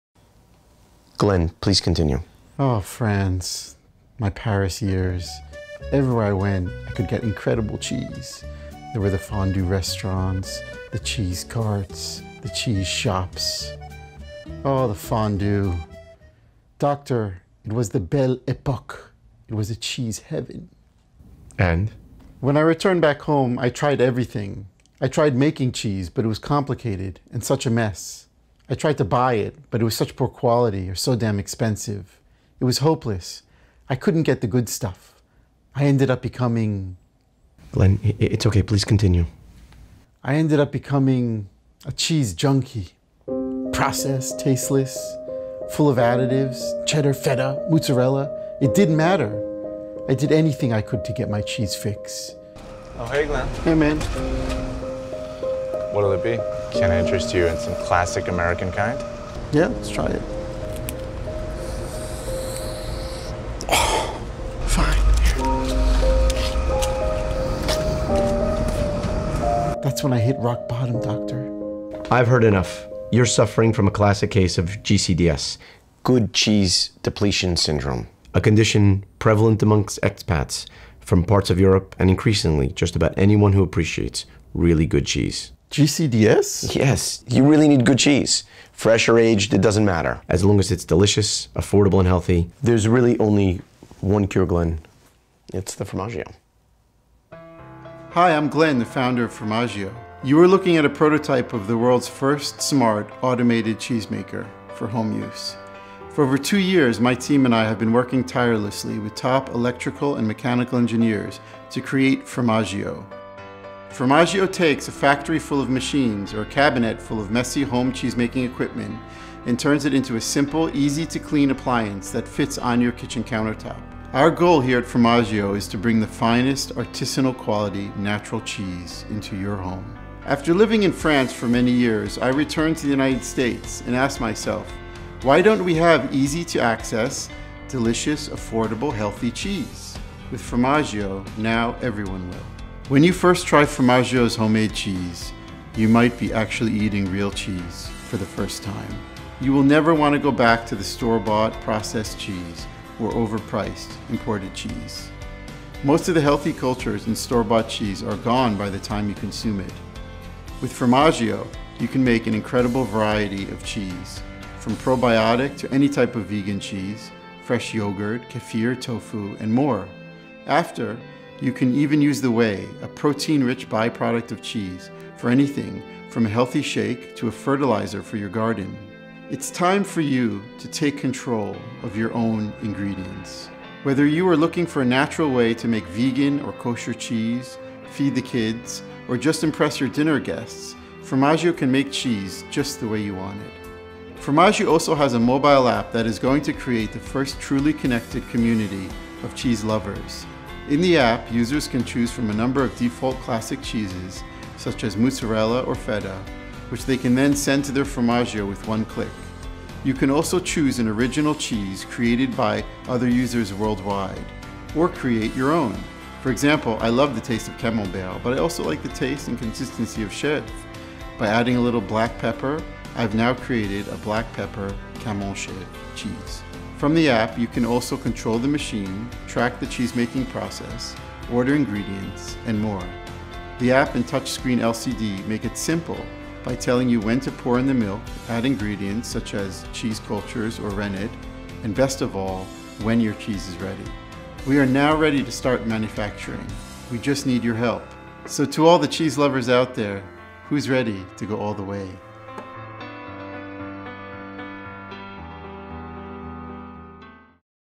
FROMAGGIO The World's first Smart Home Cheese maker
A smart, automatic cheese maker that takes technology from advanced factory machines and combines it with the artisanal aspect and convenience of at-home cheese making. With Fromaggio, cheese is more authentic, natural and healthier than store bought cheese.

Please note that some of the links above are affiliate links, I will earn a commission if you decide to make a purchase after clicking through the link at no additional cost to you.